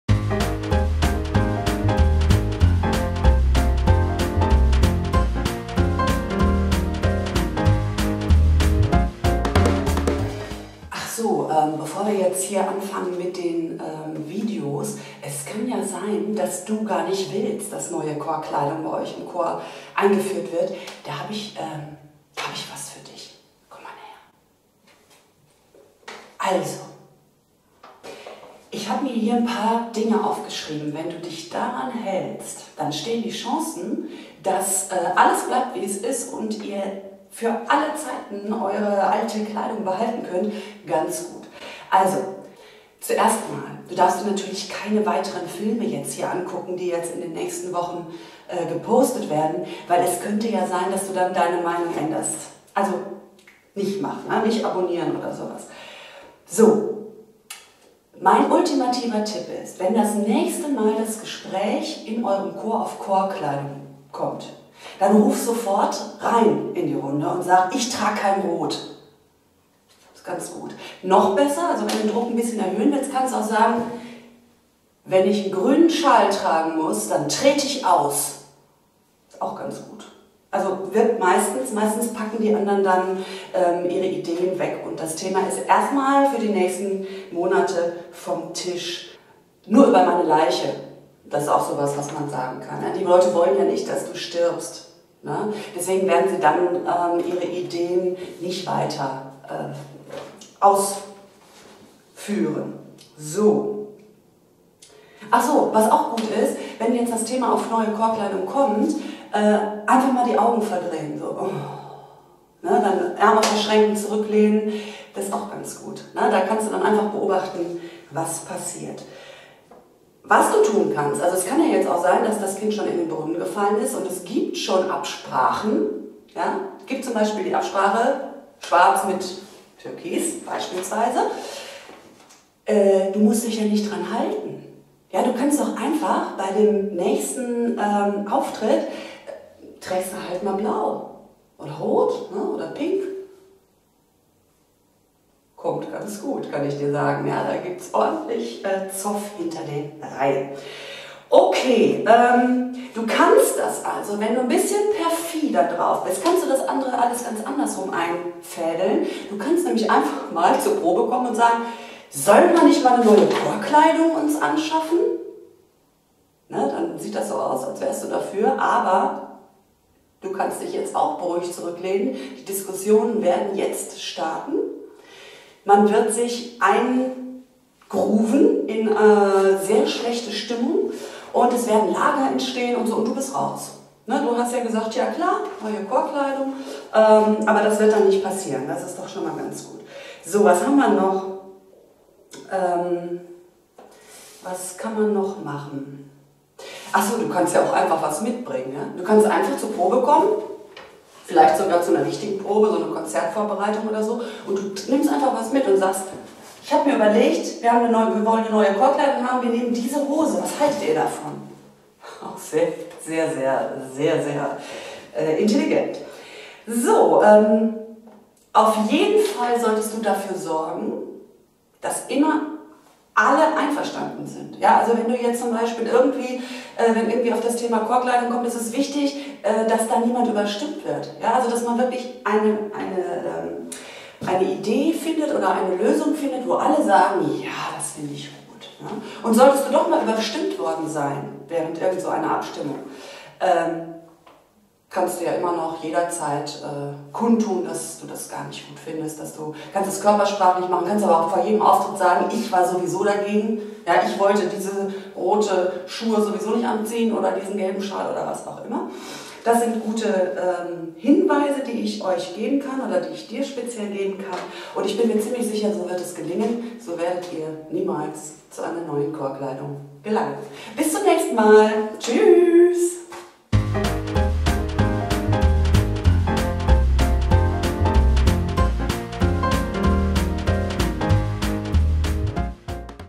Neue Chorkleidung gekonnt verhindern
Über Tricks und Kniffe, wie man neue Chorkleidung gekonnt verhindern kann... :-)

Leider haben wir die Produktion und Beratung zur Chorkleidung wegen der Corona-Pandemie einstellen müssen. Alle Inspirationen und Ideen findet ihr aber nach wie vor auf unserer Seite: